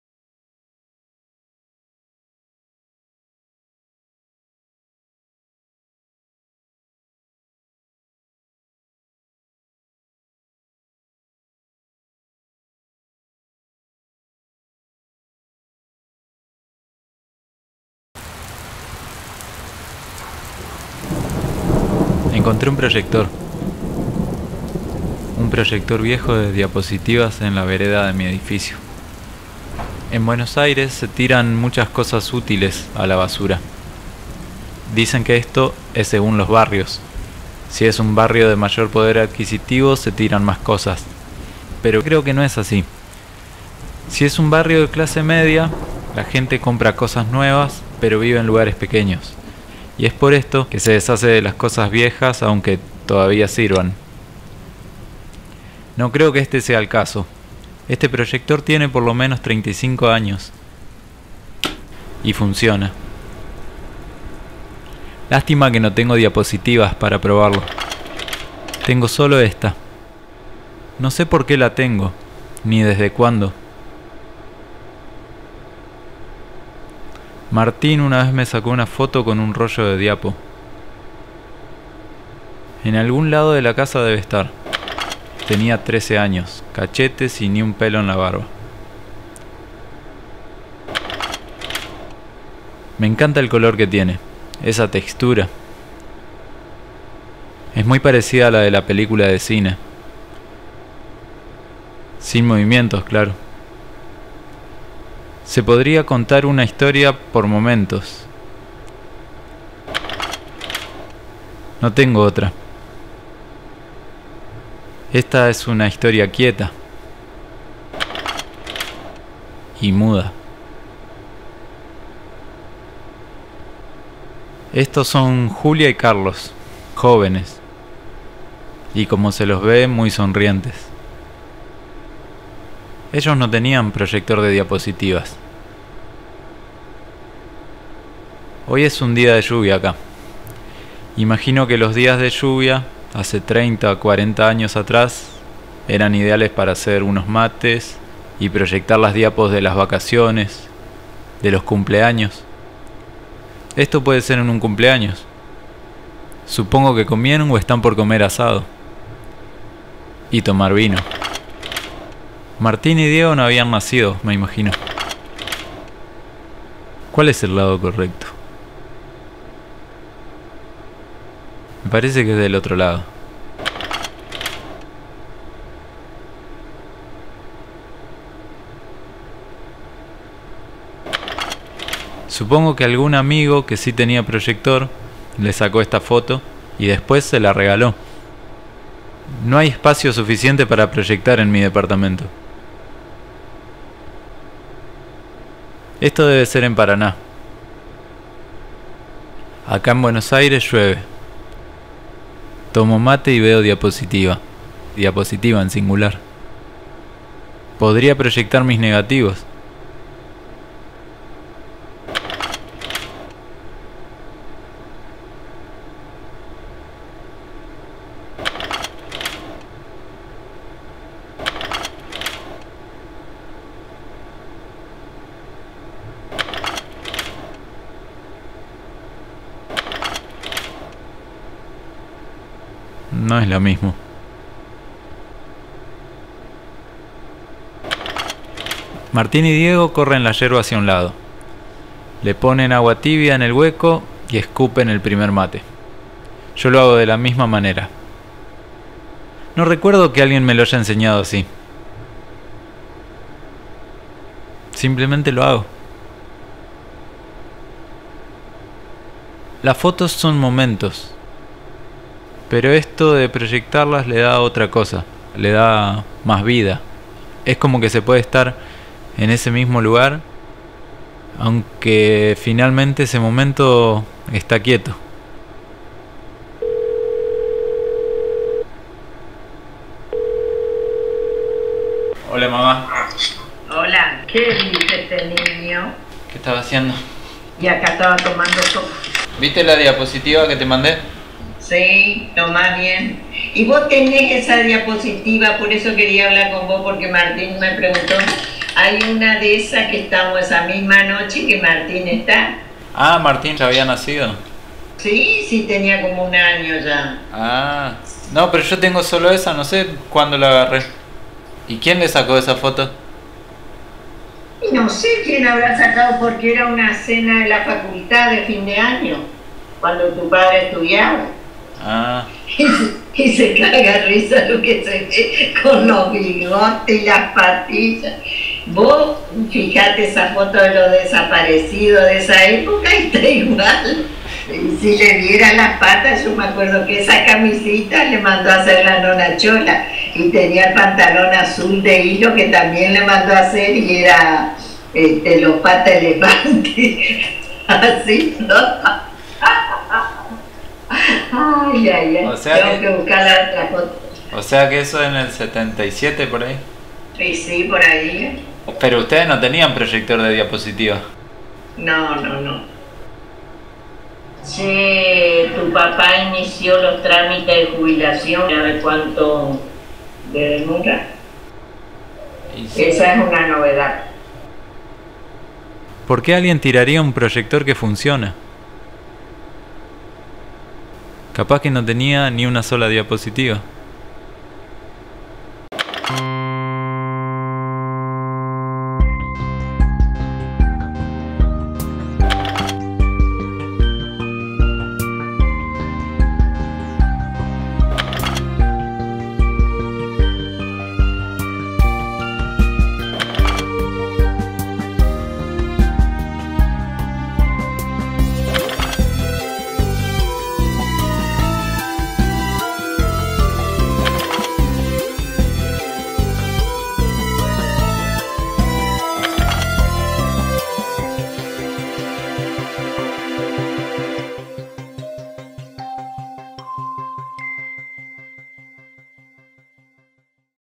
Una diapositiva - Cortometraje
Encontré un proyector de diapositivas.
Las fotografías son momentos, estos pueden volver a ser vividos? Una diapositiva dice tanto.
¿Porqué alguien tiraría un proyector de diapositivas?

I found a slide projector.
Photos are now. These moments can be lived again?
One slide says so much.
Why would anyone throw a slide projector?

Original music: Emiliano Rios.
Director and producer: Mariano Graglia.